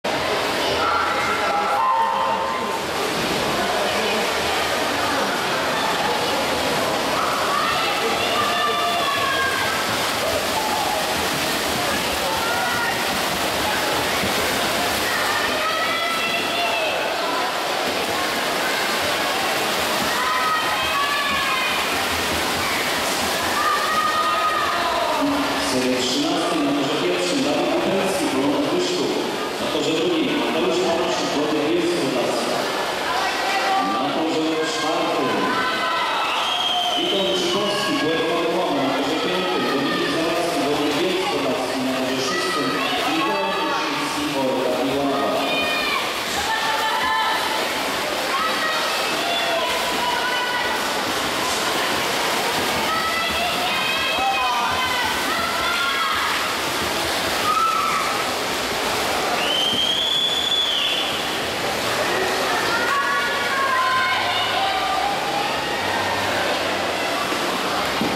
Mat. Panasiuk (2) i D. Zawadzki (5), 100 m dowolnym
Ogólnopolskie Zawody Pływackie o Puchar Burmistrza Miasta Mława, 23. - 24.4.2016